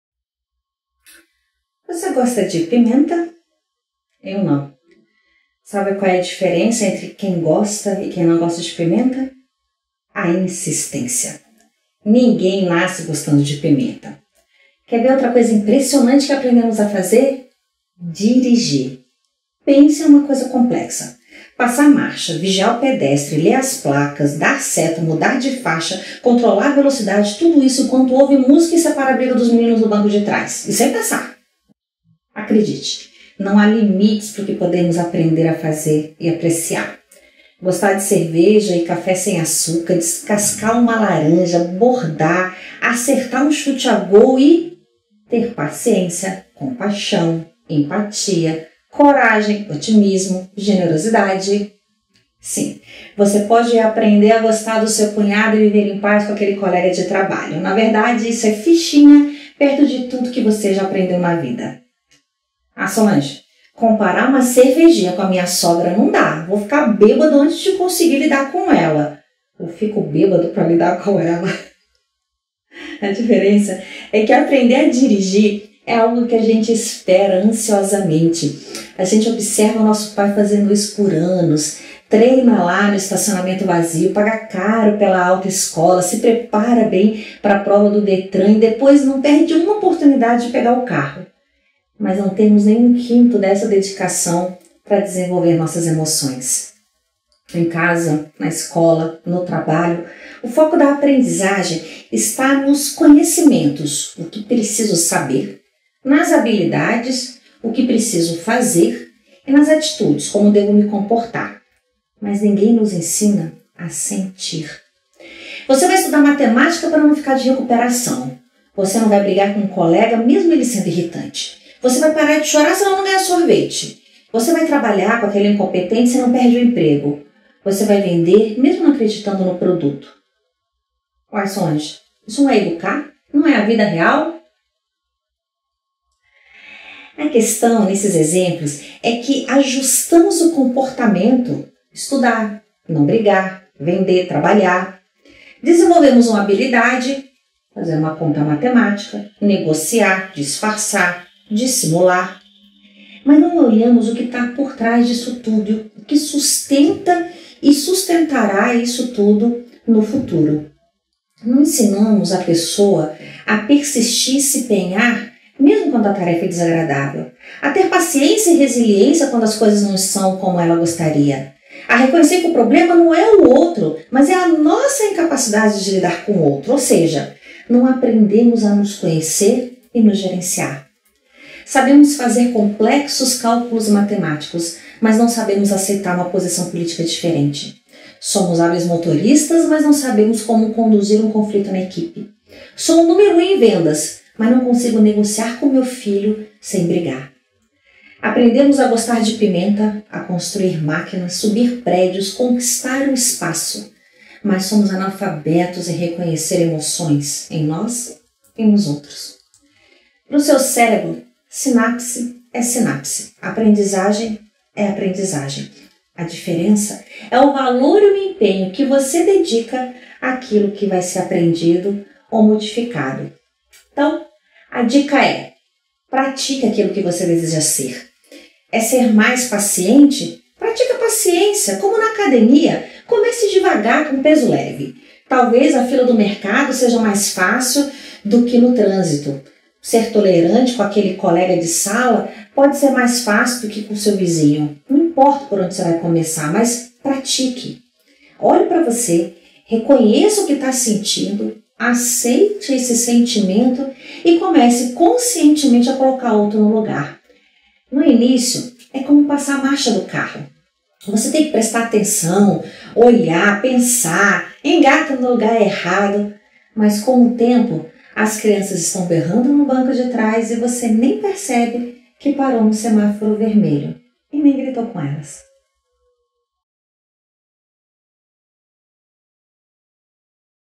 Você gosta de cerveja?
Qual a relação que existe entre você gostar de cerveja e sua falta de paciência? Pois saiba que uma cervejinha bem gelada pode te ensinar muito sobre tolerância, compaixão, empatia, coragem, otimismo, generosidade...